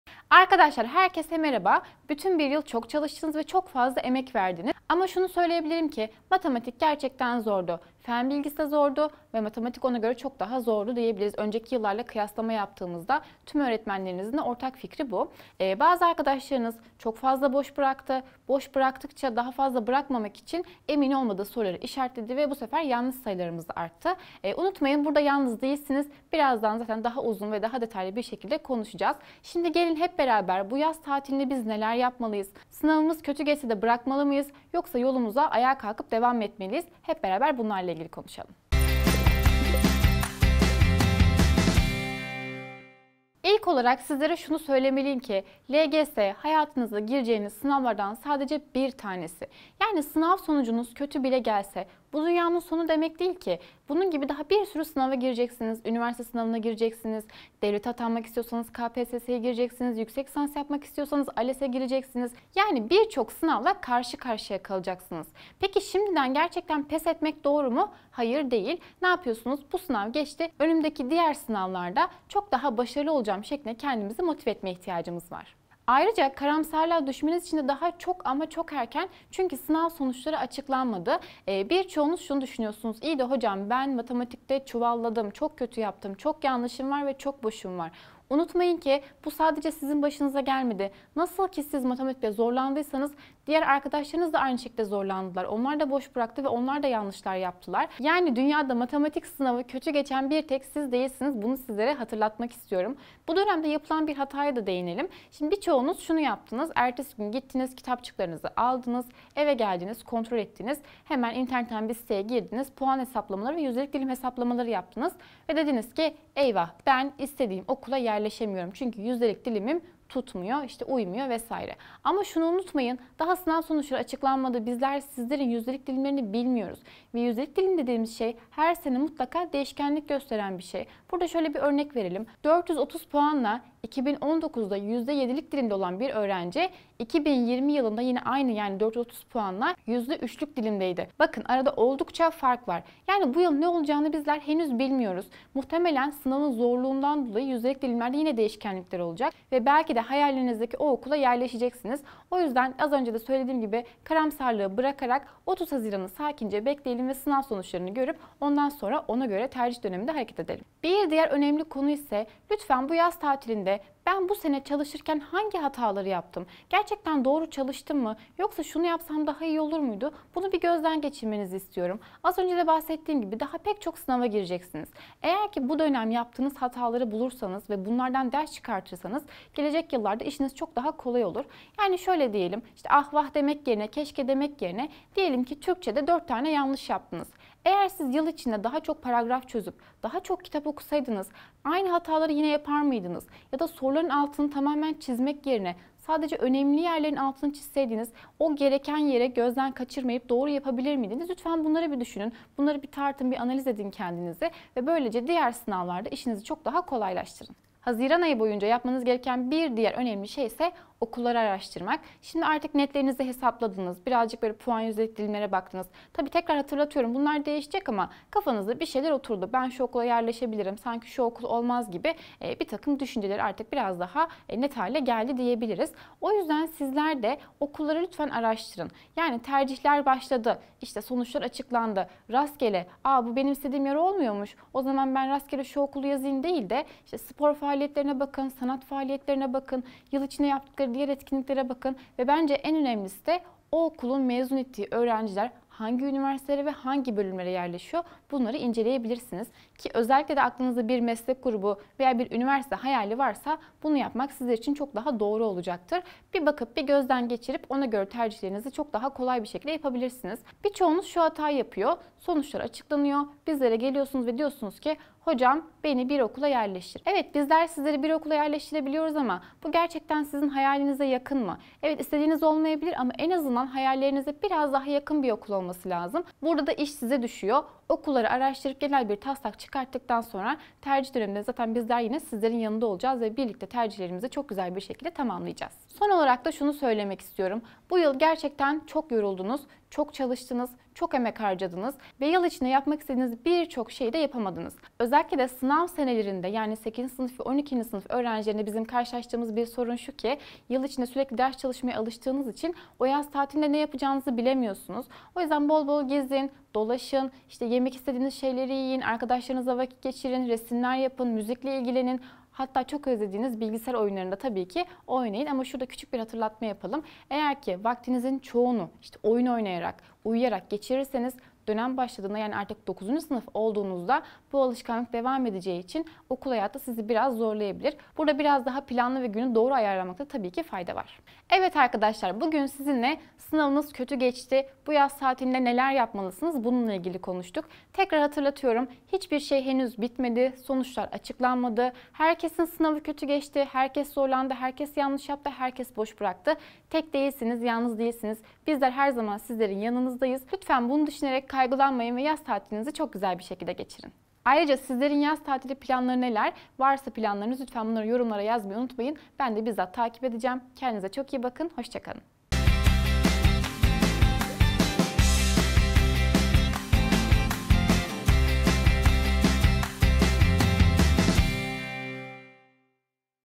LGS İstediğim Gibi Geçmedi, Şimdi Ne Yapacağım?
LGS istediğin gibi geçmemiş olabilir. Ama önemli olan bundan sonra ne yapacağımız. Bu videomuzda LGS sonrasında yapmamız gerekenleri anlatıyoruz. 🧐